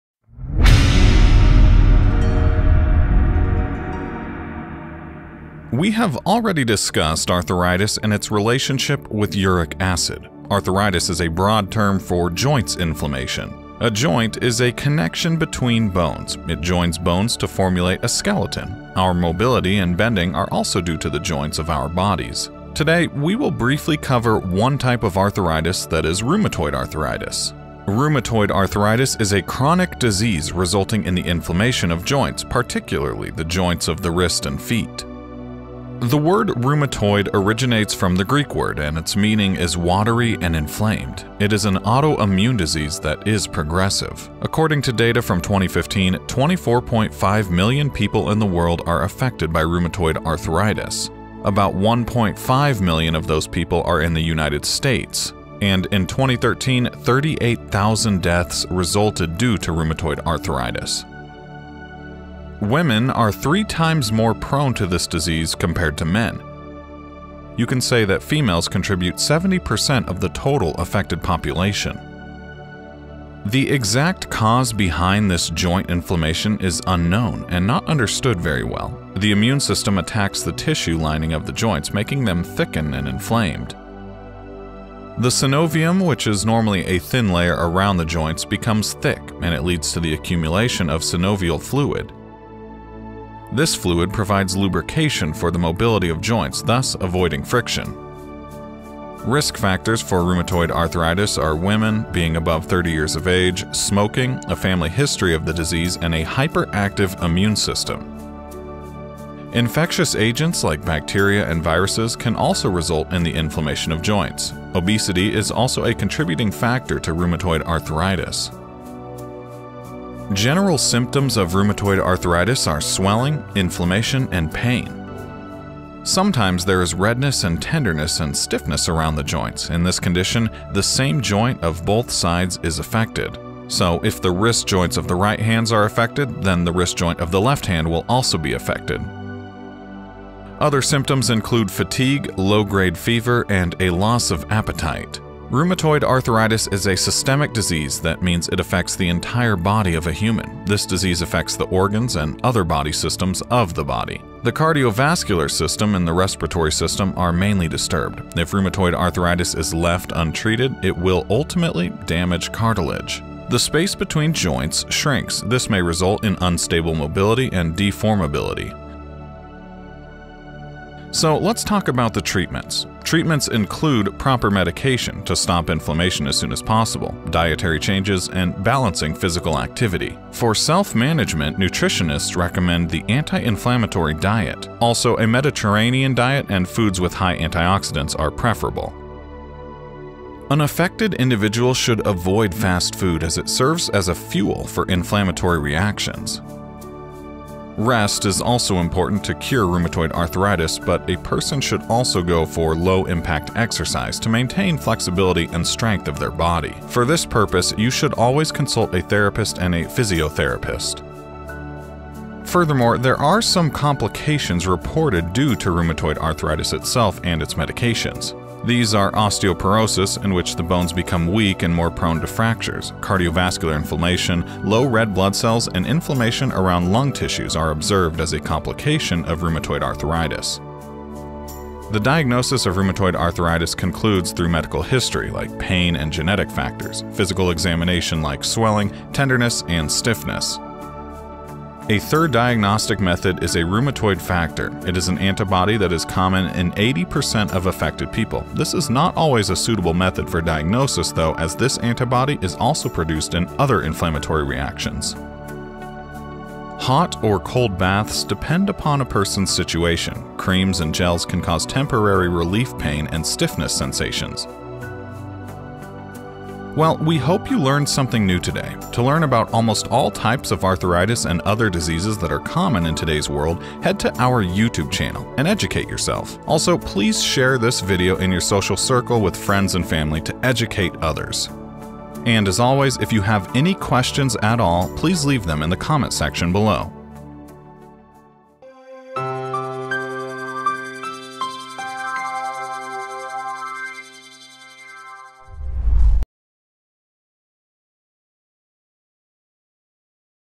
What is Rheumatoid Arthritis
A joint is a connection between bones; it joins bones to formulate a skeleton. Our mobility and bending are also due to the joints of our bodies.
Today we will briefly cover one type of arthritis that is rheumatoid arthritis. Rheumatoid arthritis is a chronic disease resulting in inflammation of joints, particularly joints of wrist and feet.
 The word rheumatoid originates from the Greek word, and its meaning is watery and inflamed. It is an autoimmune disease that is progressive.
According to the data of 2015, 24.5 million people in the world are affected by rheumatoid arthritis. About 1.5 million people in the United States are affected by rheumatoid arthritis. In 2013 38,000 deaths resulted due to  Rheumatoid Arthritis.

Women are three times more prone to this disease as compare to men. You can say that females contribute 70% of the total affected population.
The exact cause behind this joint inflammation is unknown and not understood very well. Immune system attacks on the tissue lining of joints, making them thicken and inflamed.
The Synovium, which is normally a thin layer around joints becomes thick, and it leads to the accumulation of synovial fluid. This fluid provides lubrication for motility of joints thus avoiding friction.